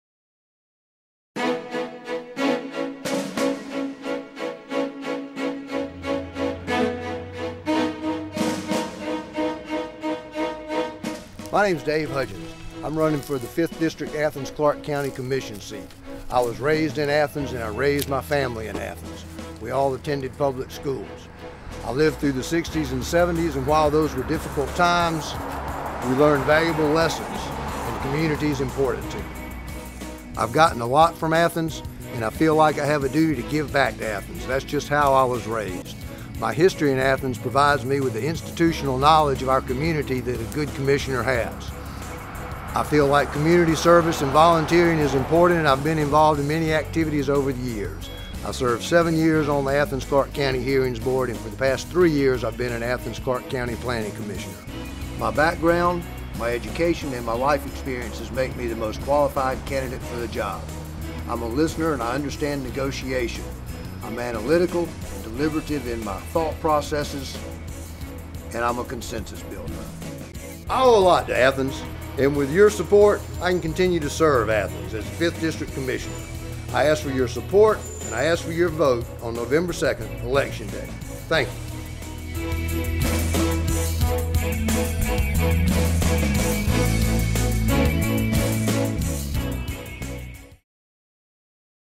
Elect Dave Hudgins - 5th District Commissioner
Dave Hudgins is running for the Athens-Clarke County 5th District Commissioner seat. Here Dave discusses his background and qualifications that make him the best candidate for the job. Election Day is upon us, and Dave would greatly appreciate your support and your vote!